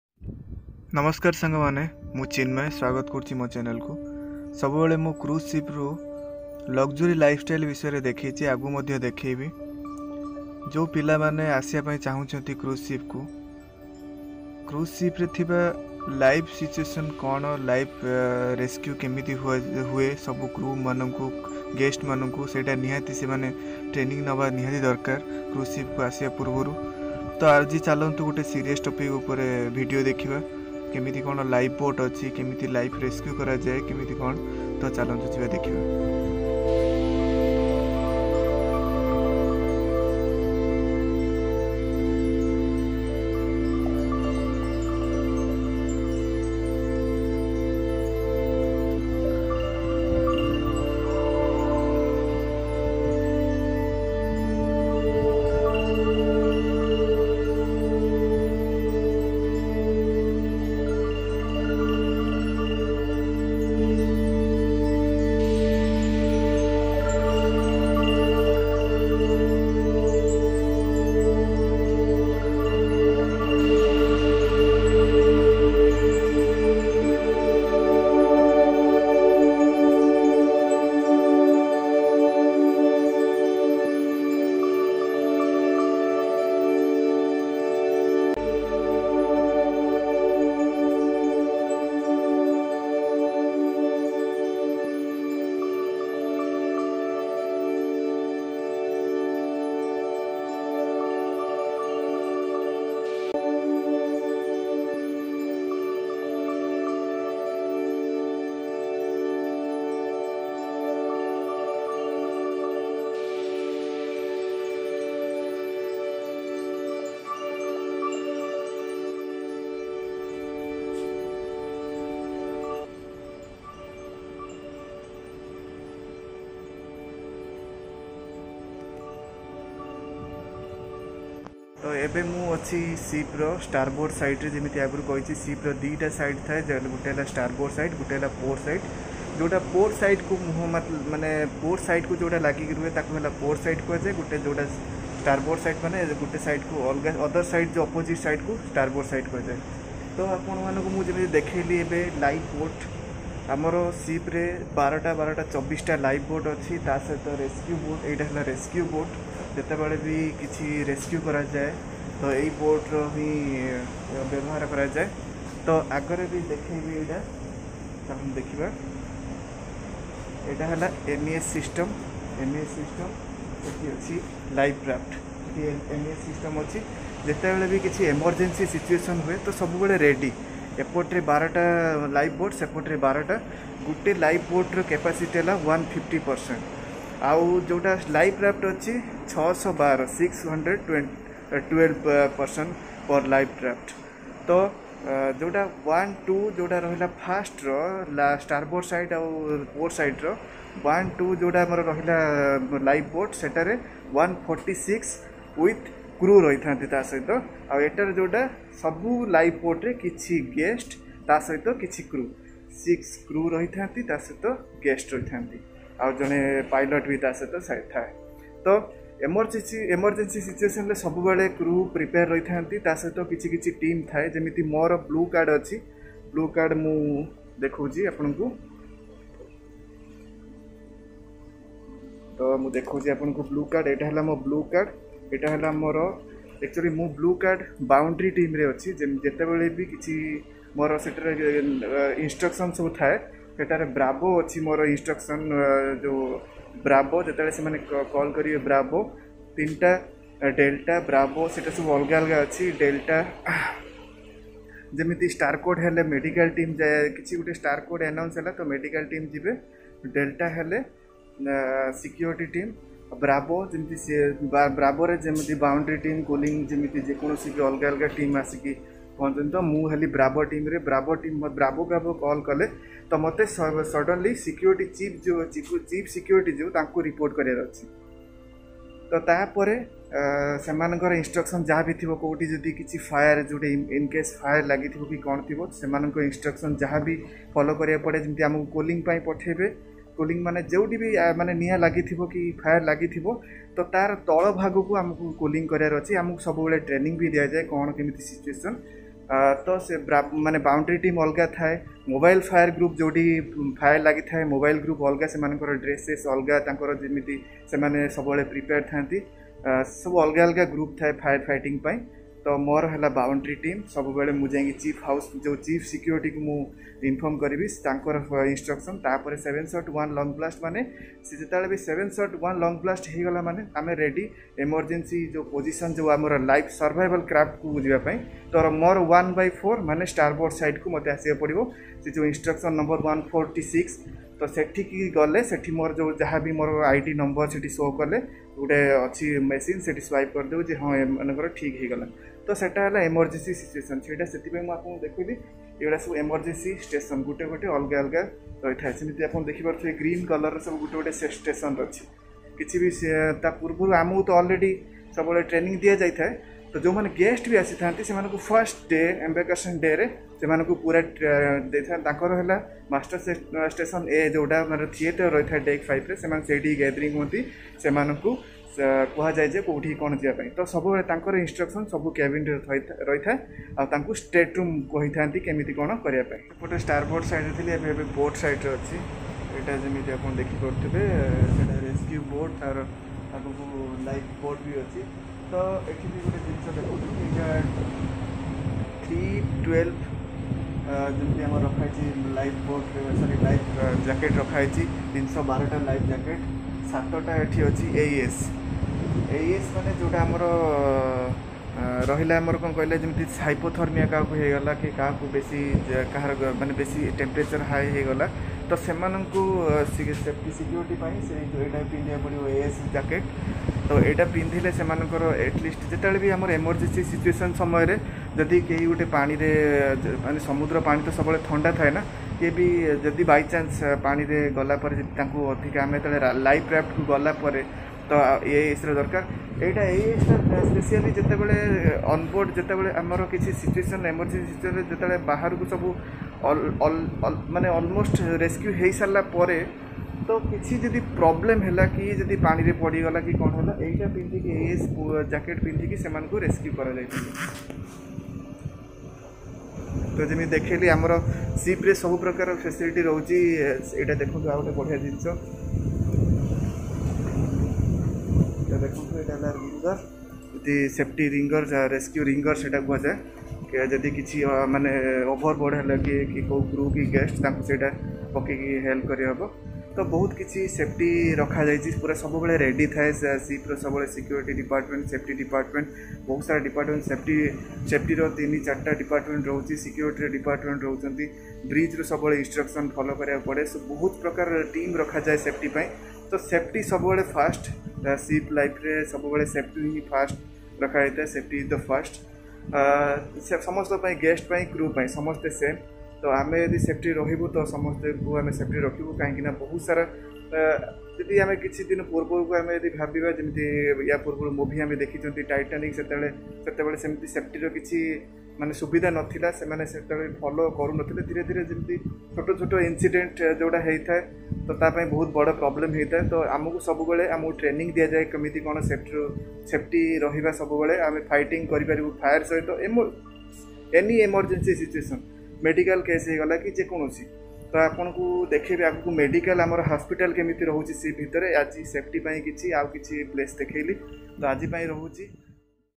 Odia Vlog ବଡ଼ ବଡ଼ ଜାହାଜ ରେ emergency situation ରେ କଣ କରାଯାଏ🙄🙄😱 ? emergency situation on cruise ship
Cruise ship ରେ emergency situation ରେ କଣ କରାଯାଏ | cruise ship emergency situation | Odia Vlog

ଚ୍ୟାନେଲ ଟି ଓଡିଶାର ପ୍ରଥମ ଓଡ଼ିଆ Cruise Ship Vlogs ଚ୍ୟାନେଲ ଯେଉଁଥିରେ ଆପଣ ମାନେ Cruise Ship ରେ କଣ lifestyle ଥାଏ ଦେଖିପାରିବେ। ଧନ୍ୟବାଦ୍।

Today I am showing my cabin tour "a home away from home" vlog in cruise ship in odia language.